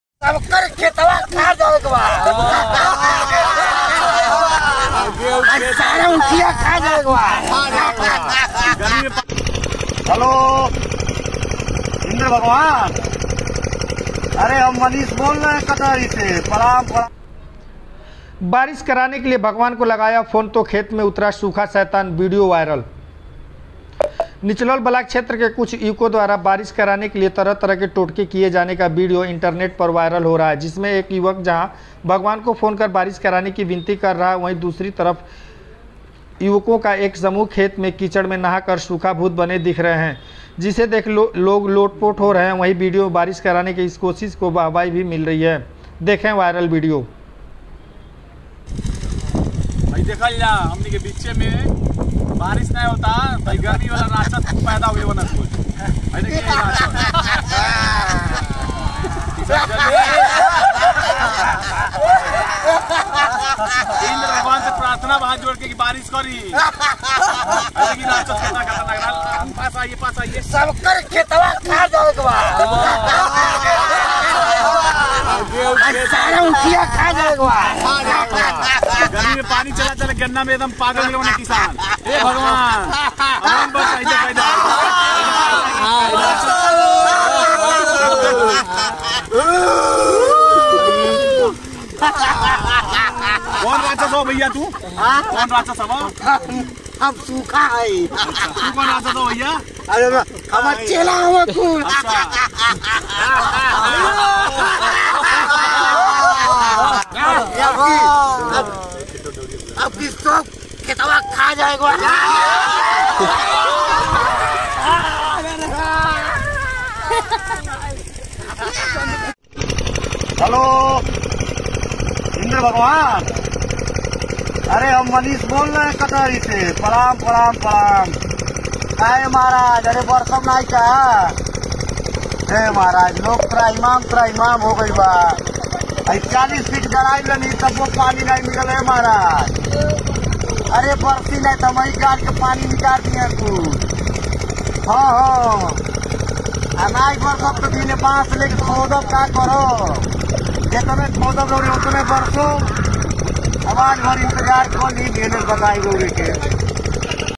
महराजगंज : बारिश कराने के लिए भगवान को लगाया फोन, खेत में उतरे सुखा शैतान, विडियो वायरल
महराजगंज : बारिश कराने के लिए भगवान को लगाया फोन तो खेत में उतरे सुखा शैतान, विडियो वायरल

साथियों दैनिक संवाद न्यूज़ चैनल में आपका स्वागत है। 💐💐